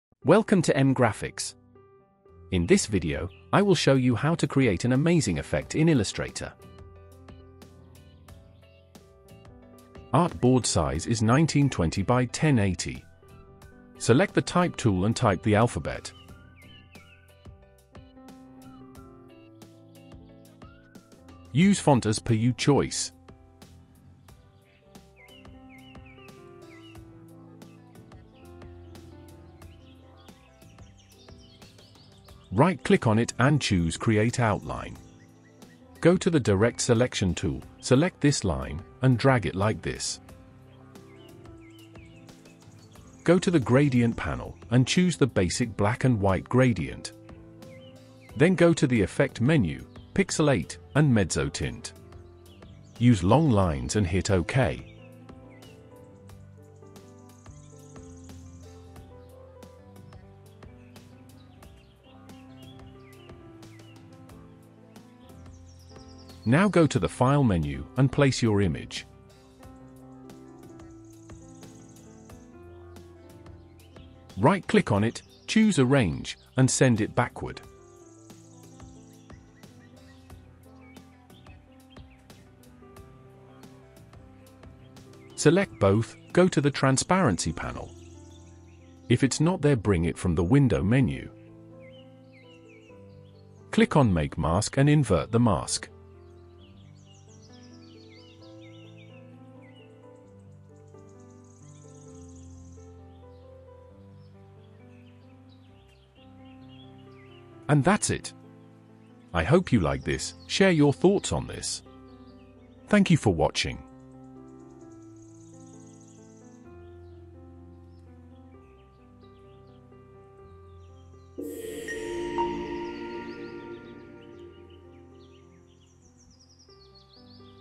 Create an Amazing effect in Adobe Illustrator | Mezzotint Effect | Illustrator Tutorials
Welcome to M graphics
In this tutorial, I will teach you How toCreate an Amazing  effect in Adobe Illustrator | Mezzotint Effect |  Illustrator Tutorials | M Graphics.

illustrator tutorial,
adobe illustrator,

M Graphics YouTube Channel...